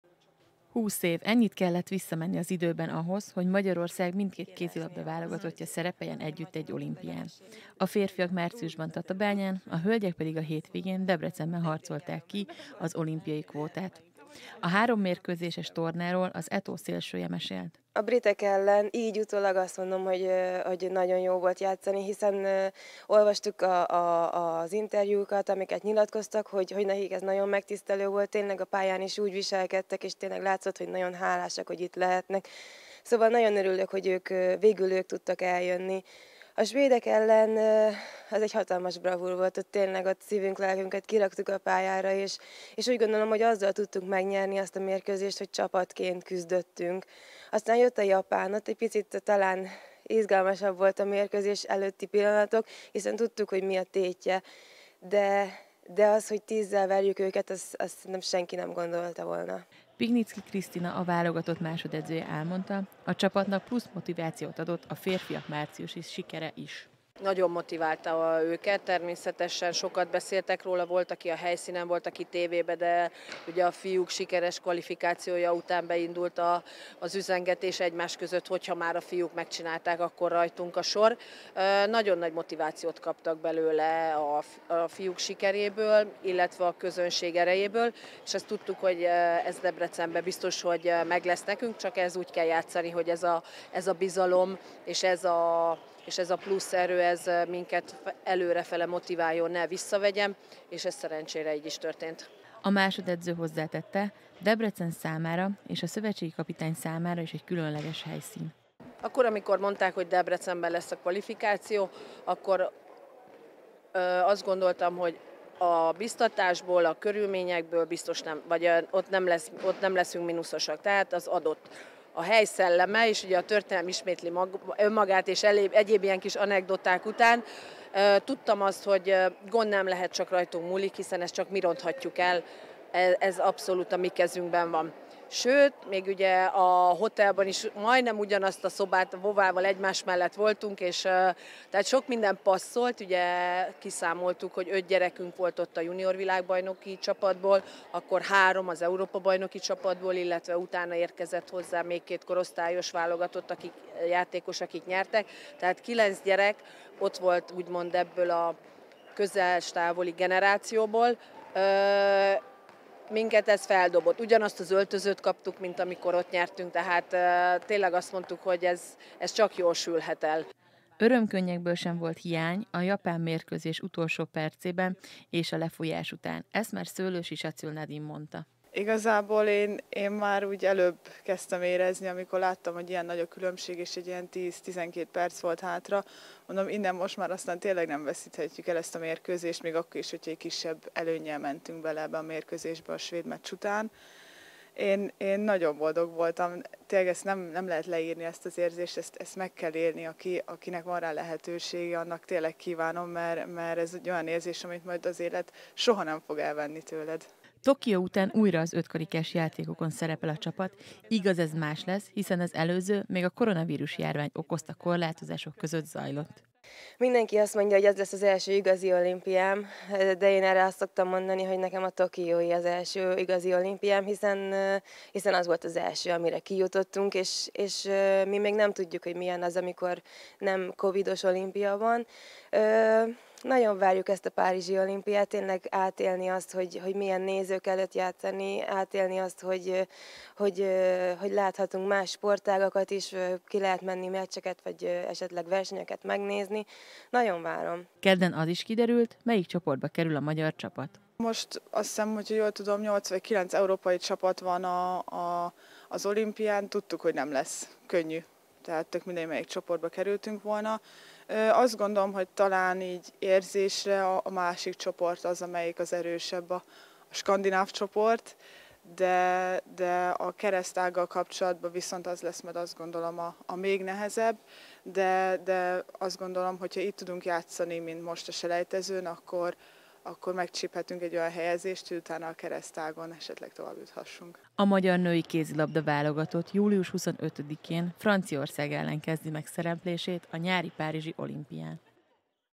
Olimpiai kézi-kvóta győriekkel
A tokiói olimpia után újra a ötkarikás játékokon mérheti össze tudását a magyar női kézilabda-válogatott, miután a hétvégén megnyerte a selejtezőtornát Debrecenben. Golovin Vlagyimir együttese a három mérkőzésből álló tornát hibátlanul zárta, így megváltotta repülőjegyét a nyári párizsi olimpiára. A sikernek győri főszereplői is voltak, hiszen a Győri Audi ETO KC együttesét két szakember és két játékos is képviselte a nemzeti együttesben.